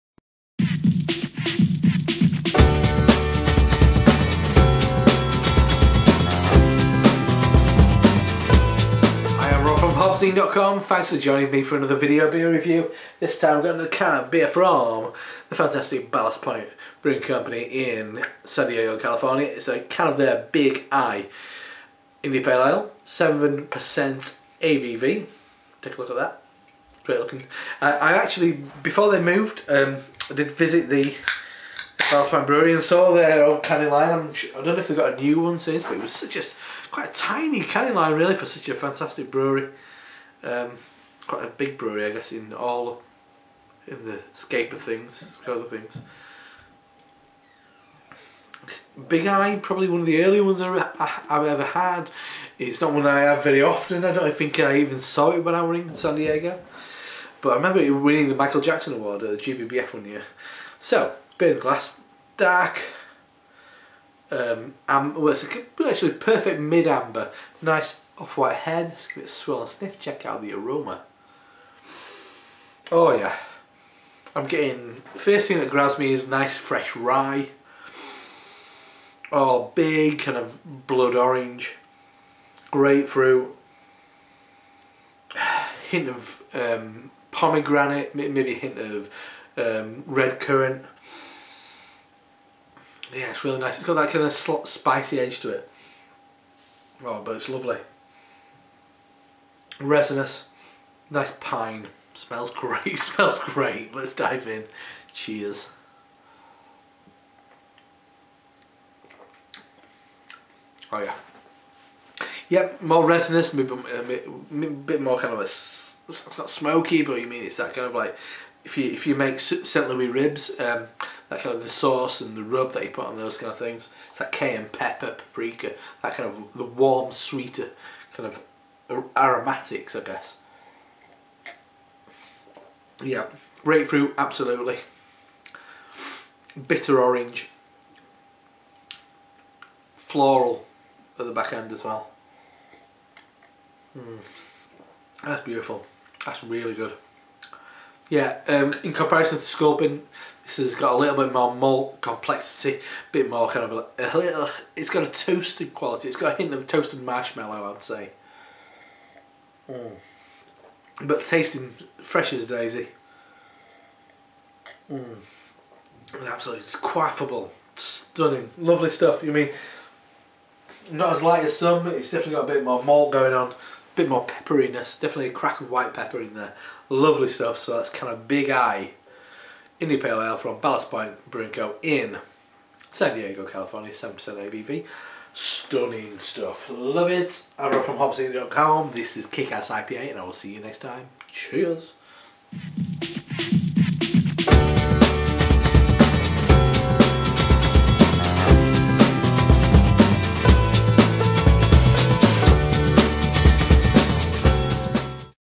Ballast Point Brewery - Big Eye (IPA) - HopZine Beer Review
Thanks for joining me for another video beer review. This time I'll be drinking a bottle of Ballast Point's Big Eye IPA.